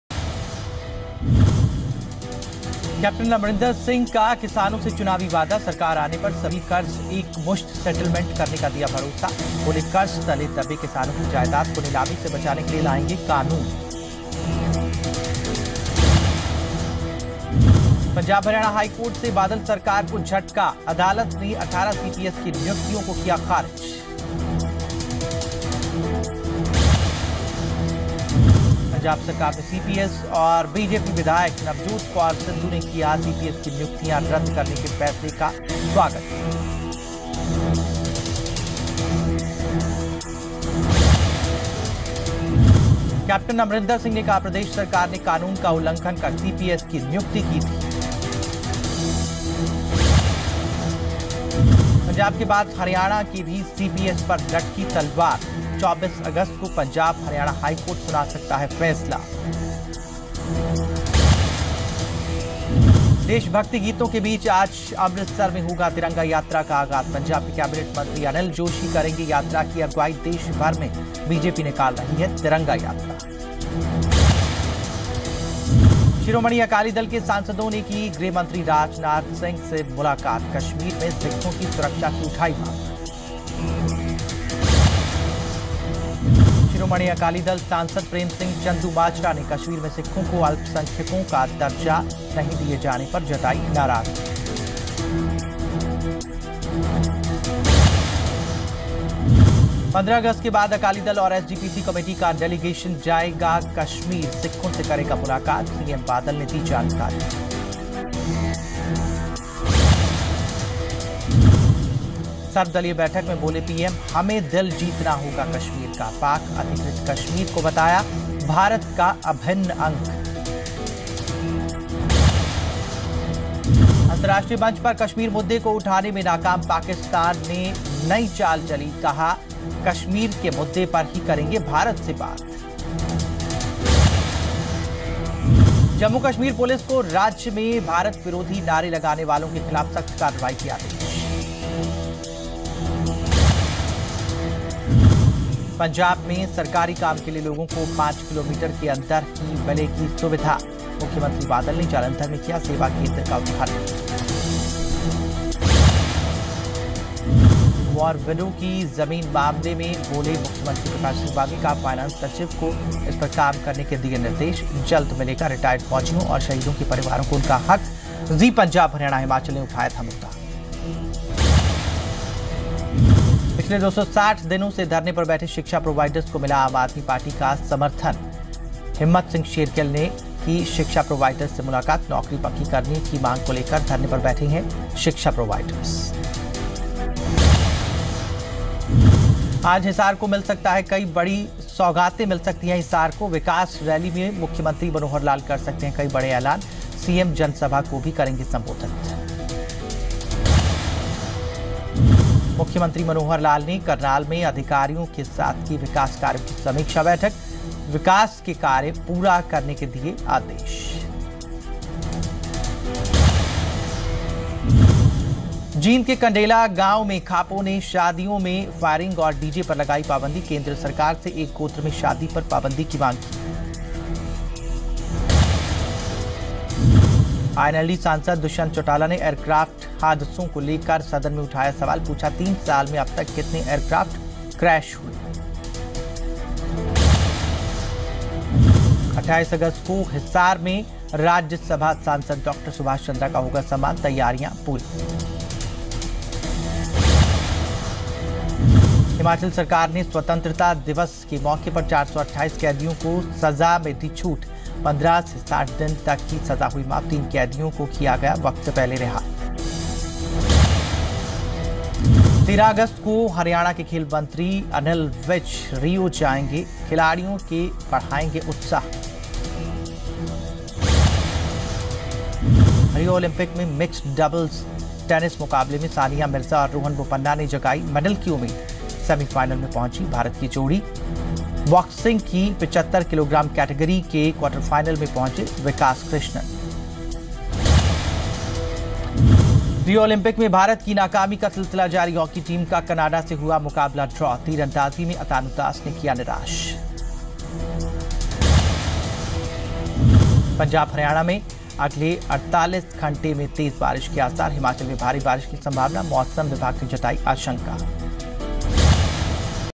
Captain Amarinder Singh's love for farmers before the elections
Captain Amarinder Singh promises to settle the farmer's loan after the elections. To know more watch the full news story.